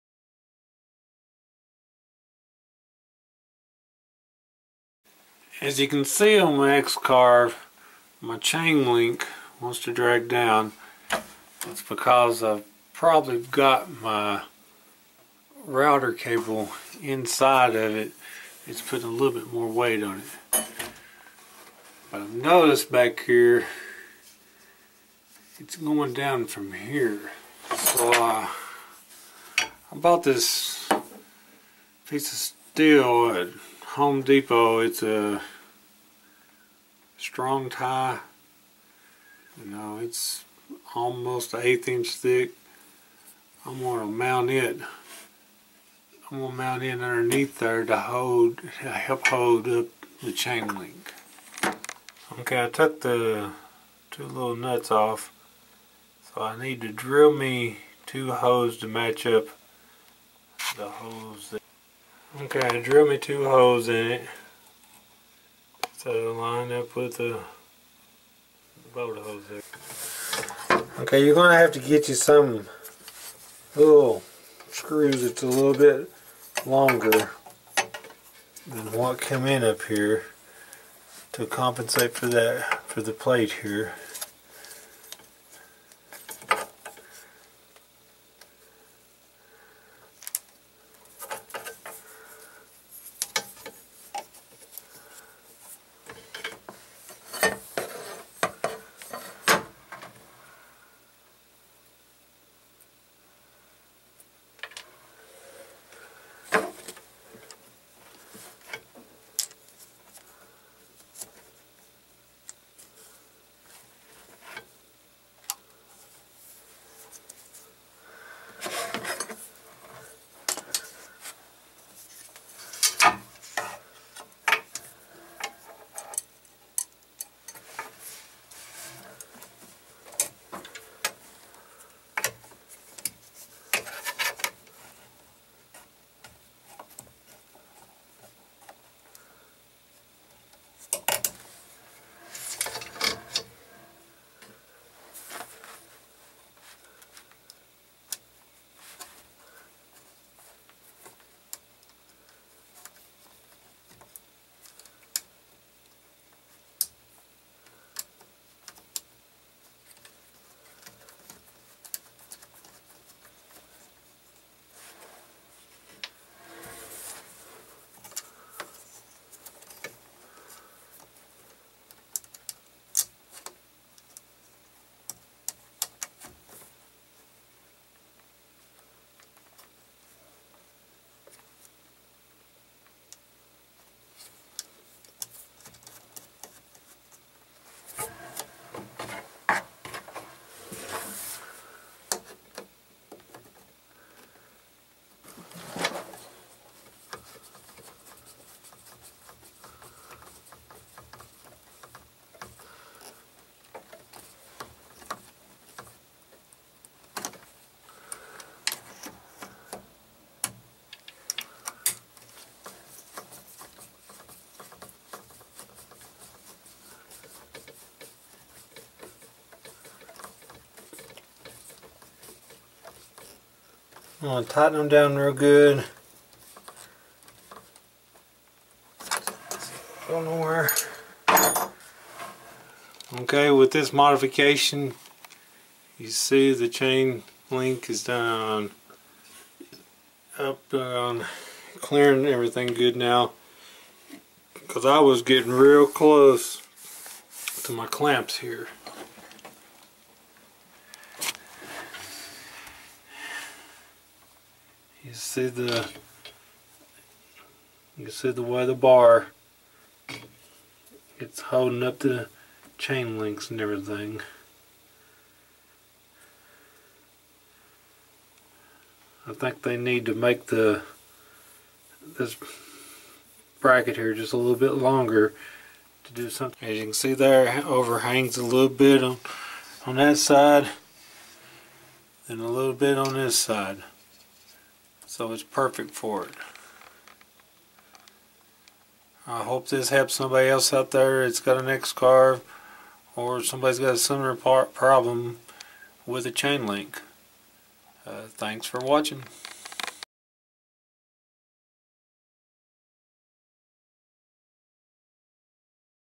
X-Carve CNC Router Modifying Drag Chain or Chainlink
In this video ,I show how I modified my Drag Chain-link, on my X-Carve CNC Router.  Using a piece of steel from Home Depot